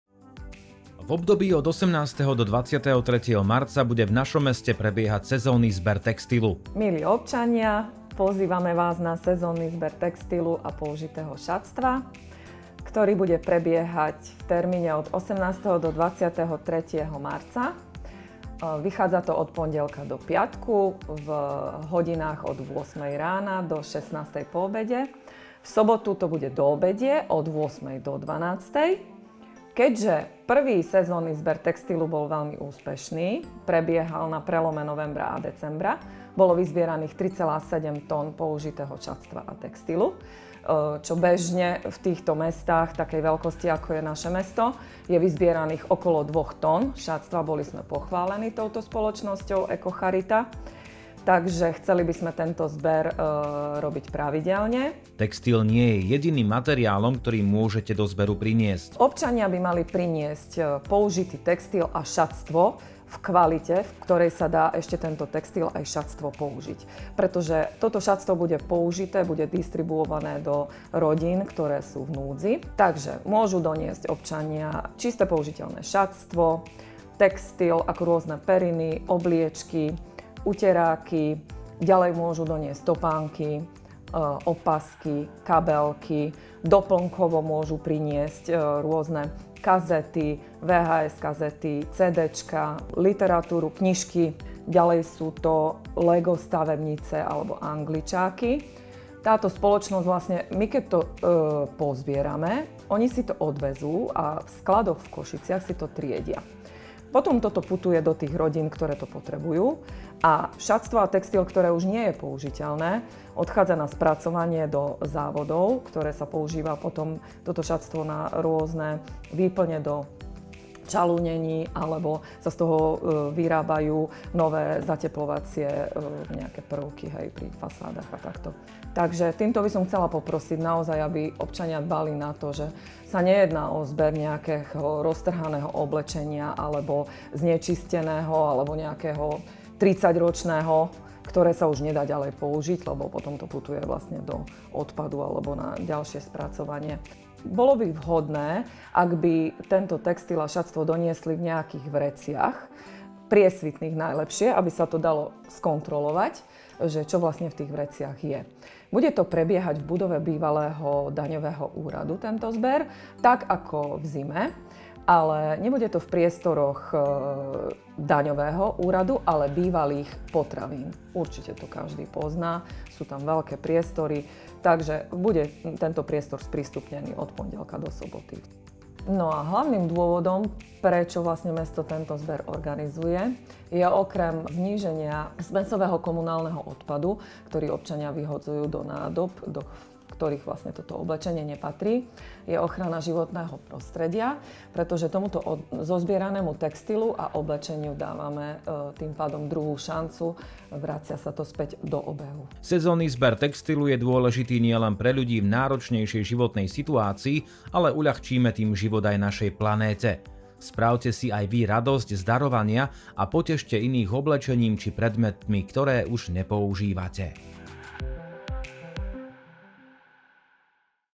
V Moldave sa opäť uskutoční sezónny zber textilu
V období od 18. do 23. marca bude v našom meste prebiehať sezónny zber textilu.